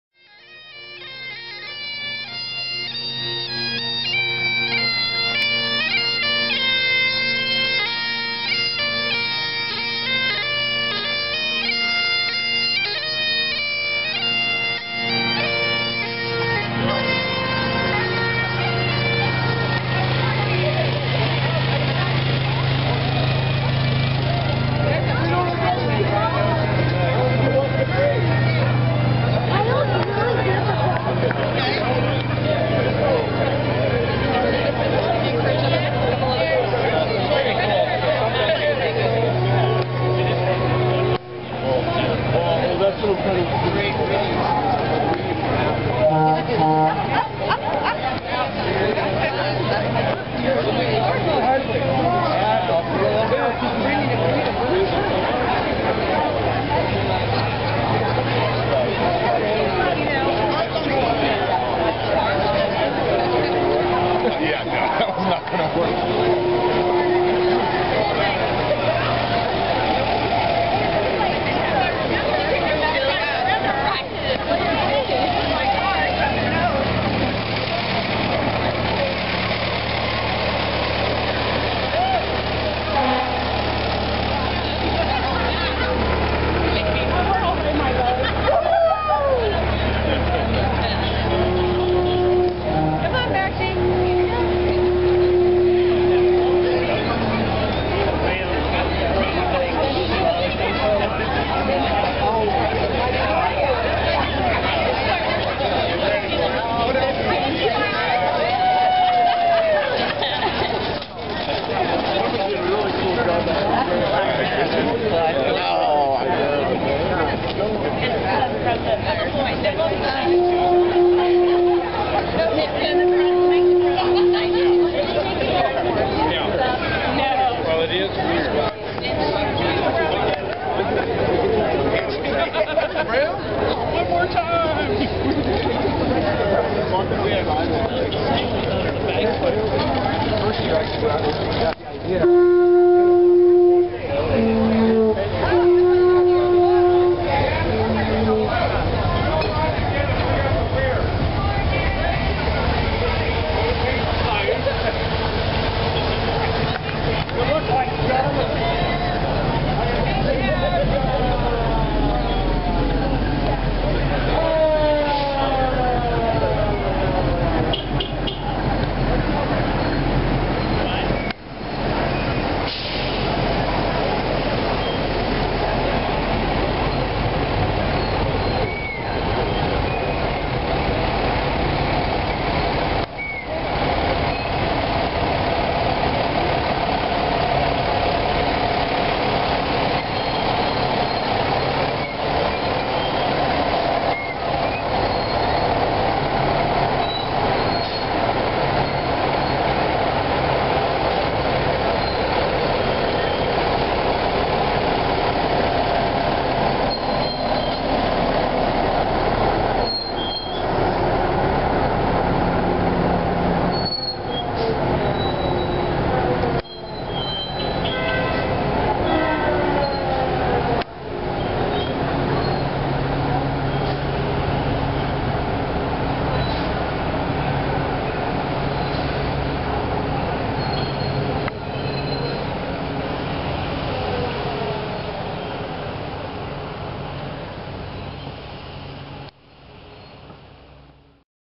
Green Hair Parade, Healdsburg 2011
Every St. Patrick's Day for the past 22 years, the patrons of the B&B Saloon and Lounge gather at daybreak to hoist a few, toast each other's dyed hair, then perambulate around the Plaza in a self-congratulatory haze. Shot with a Canon Powershot A590, standing in one place, watching life's parade pass by.